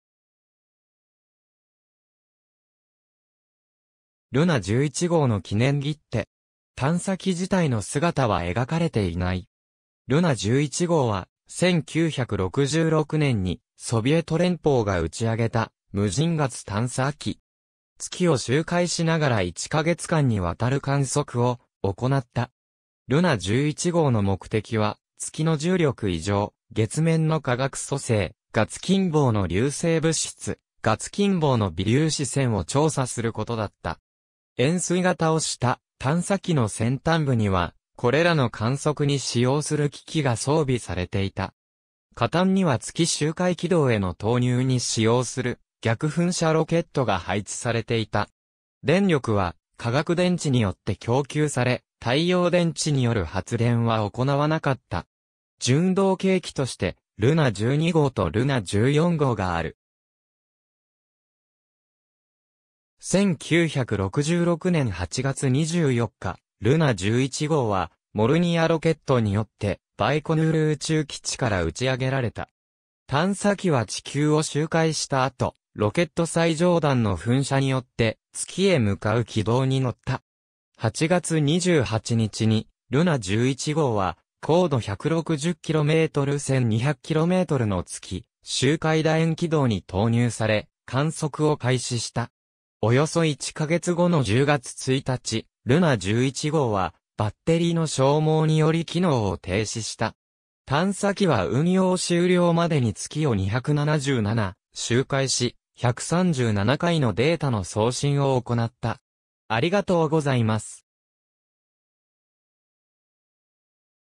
ルナ11号
ルナ11号の記念切手（1966年、ソ連）。
探査機自体の姿は描かれていない。
ルナ11号（ロシア語：Луна-11、ラテン文字表記の例：Luna 11）は、1966年にソビエト連邦が打ち上げた無人月探査機。
月を周回しながら1か月間に渡る観測を行った。
ルナ11号の目的は、月の重力異常・月面の化学組成・月近傍の流星物質・月近傍の微粒子線を調査することだった。
円錐形をした探査機の先端部には、これらの観測に使用する機器が装備されていた。
下端には月周回軌道への投入に使用する逆噴射ロケットが配置されていた。
電力は化学電池によって供給され太陽電池による発電は行わなかった。
準同型機としてルナ12号とルナ14号がある。
1966年8月24日、ルナ11号はモルニヤロケット（8K78M）によってバイコヌール宇宙基地から打ち上げられた。
探査機は地球を周回した後、ロケット最上段の噴射によって月へ向かう軌道に乗った。
8月28日にルナ11号は高度160km×1200kmの月周回楕円軌道に投入され、観測を開始した。
およそ1ヵ月後の10月1日、ルナ11号はバッテリーの消耗により機能を停止した。
探査機は運用終了までに月を277周回し、137回のデータの送信を行った。